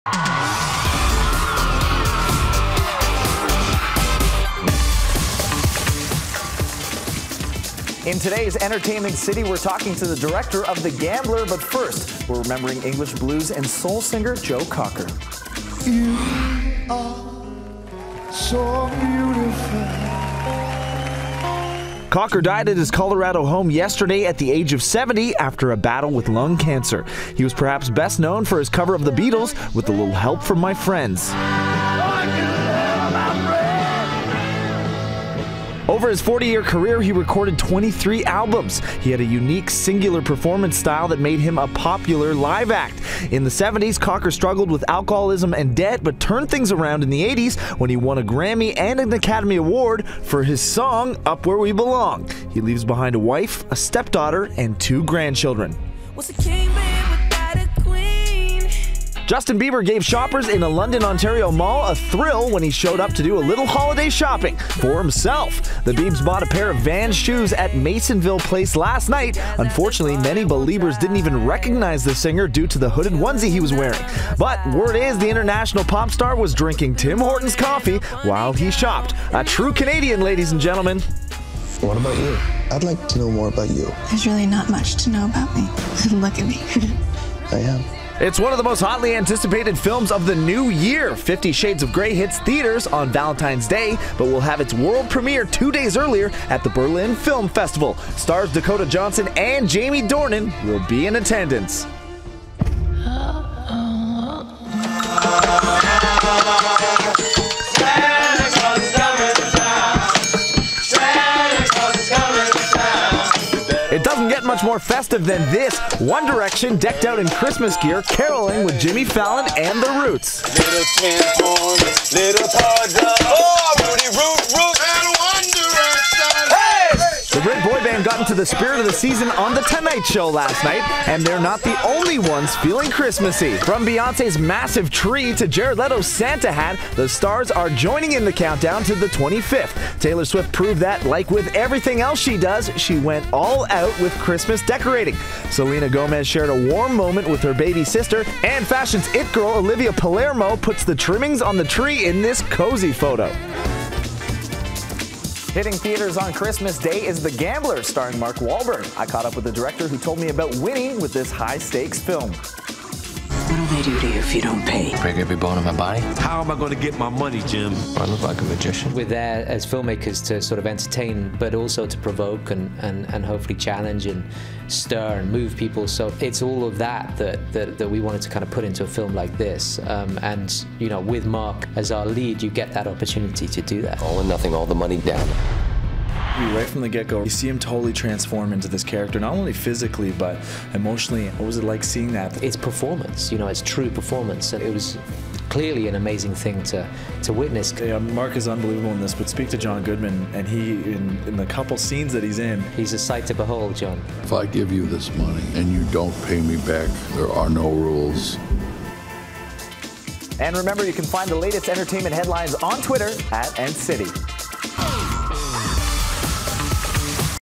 Entertainment City: Bieber shops in London, Ont., celebrities deck the halls
Justin Bieber shows up in London's Masonville mall with a Tim Hortons in hand. One Direction, Beyonce, T-Swift among the celebrities decking the halls.